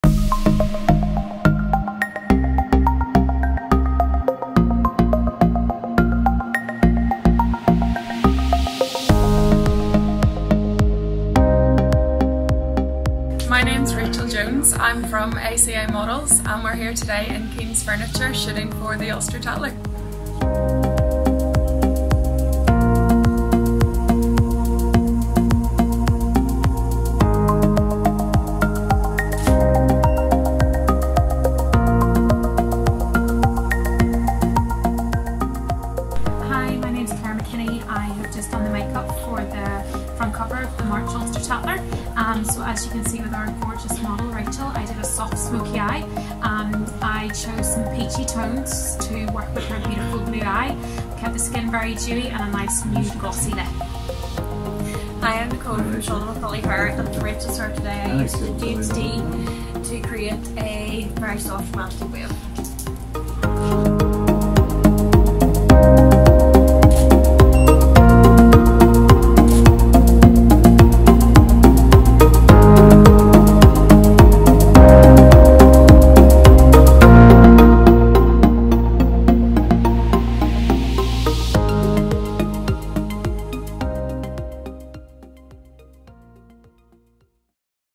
March 2019 Cover Shoot: Behind The Scenes
Here's a little look at what goes on behind the scenes at an Ulster Tatler cover shoot.

March Edition- in stores now!